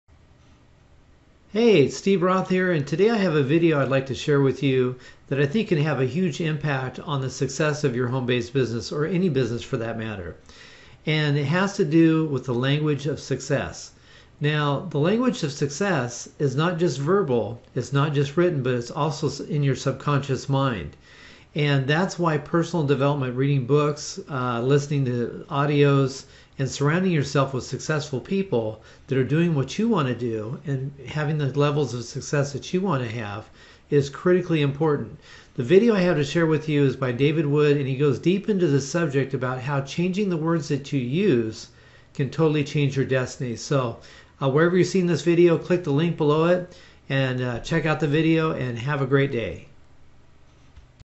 The POWER OF WORDS in Your Business - The Art Of Persuasion
The words we use to build our business either written, verbal or even subconsciously can have a huge effect on the outcome of our efforts.

New people just starting out will use language like..."I'll give it a try"...or I'd like to make a lot of money...IF this works" reveals what is already going on inside their minds.

Gain a valuable understanding in the the Art Of Persuasion as to how you can change your destiny by changing the words you use when you communicate with yourself and others.